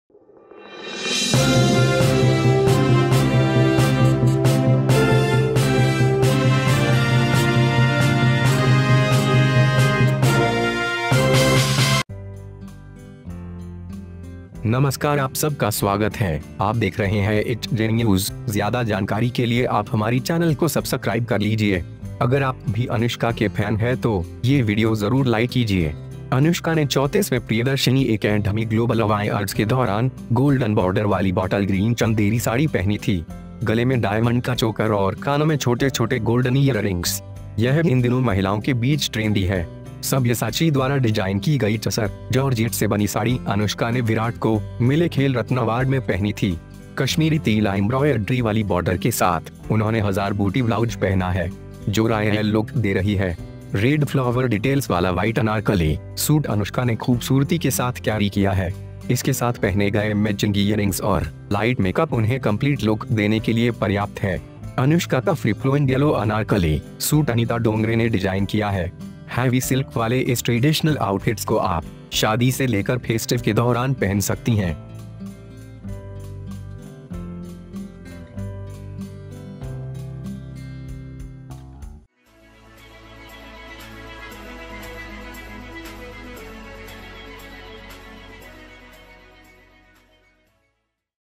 अनुष्का का देसी लुक...Anushka Desi Look
Anushka wore a golden border bottle green chanderi sari during the 34th Priyadarshini Academy Global Awards. Diamond bronze in the neck and small golden earrings in the ears. It is trendy among women these days.
Sari Anishka, made with Sister Georgette, designed by Sabyasachi, wore Virat at the Game Ratna Award. With Kashmiri tila embroidered border, he is wearing a thousand booty blouse. Which is giving the royal look.
White Anarkali suit with red flower details Anushka has beautifully carries. Matching earrings and light makeup worn with it are enough to give them a complex look.
Anushka's free floating yellow Anarkali suit is designed by Anita Dongre. You can wear these traditional outfits with heavy silk from the wedding to the festivities.

HJ NEWS !!